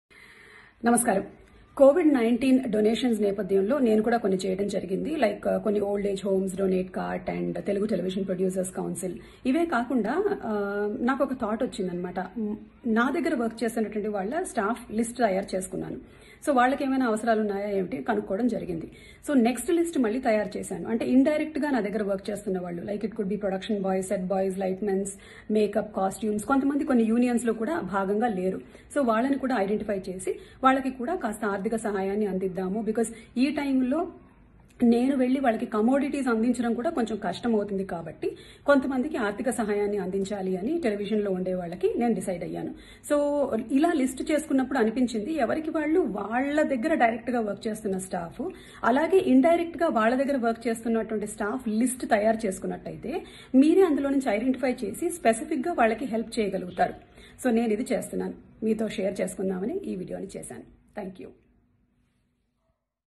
Anchor Suma Helping People out During These Hard Times || Hyderabad Today - AOne Entertainer
Hyderabad Today - AOne Entertainer aoneentertainer Anchor Suma Helping People out During These Hard Times.